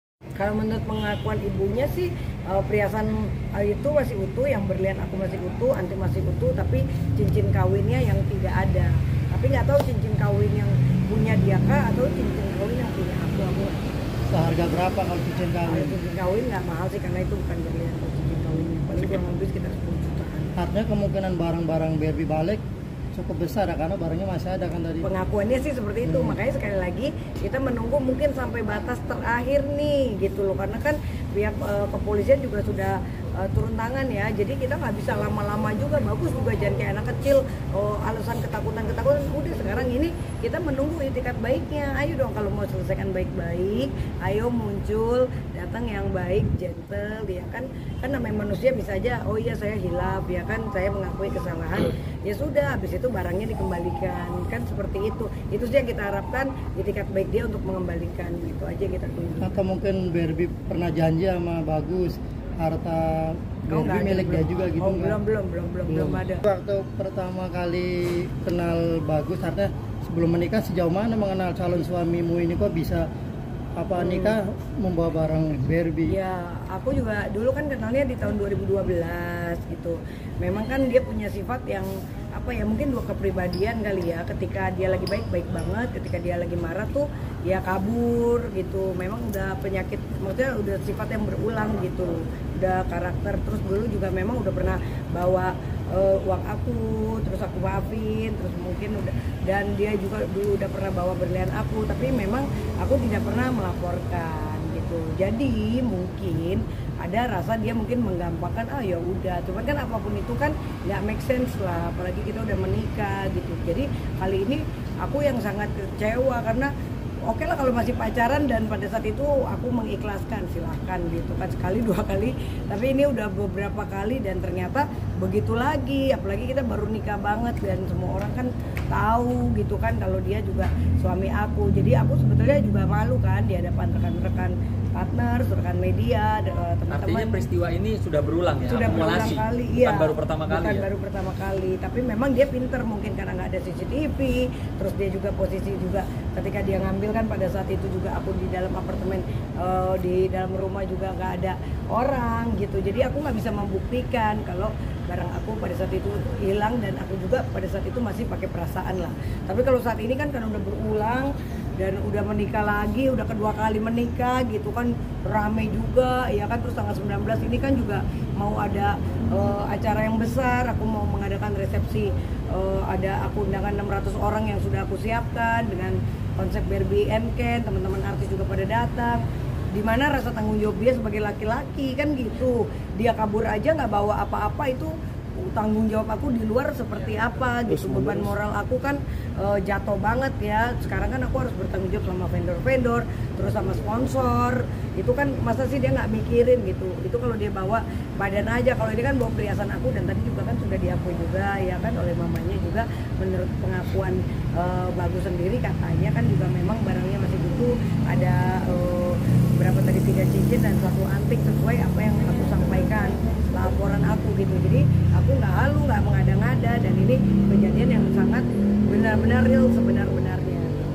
Barbie Kumalasari Kecewa Sudah Siapkan Resepsi dengan 600 Tamu Undangan
Barbie Kumalasari sebelumnya mendapat tentangan dari keluarganya untuk menikah kembali dengan Bagus Saputra. Sebelumnya, Barbie Kumalasari bercerai dengan Bagus Saputra pada 2015. Setelah itu, ia menikah dengan Galih Ginanjar, namun pernikahan ini juga berakhir dengan perceraian.

Pada 31 Mei 2024, Barbie memutuskan untuk menikah lagi dengan Bagus Saputra.

Sayangnya, belum genap satu bulan usia pernikahannya, Barbie harus menghadapi masalah karena suaminya diduga terlibat dalam pencurian emas dan berlian.

Barbie Kumalasari merasa kecewa dengan kejadian yang menimpanya saat ini. Terlebih lagi, akan ada acara resepsi yang telah direncanakan dan sudah menyiapkan 600 undangan.